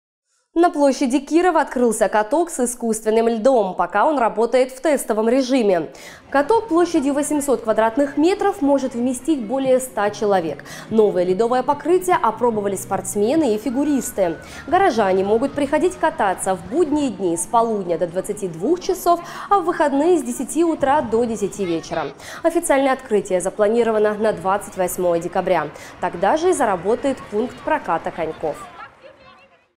Каток на площади Кирова
На площади Кирова открылся каток с искусственным льдом. Пока он работает в тестовом режиме. Каток площадью 800 квадратных метров может вместить более ста человек.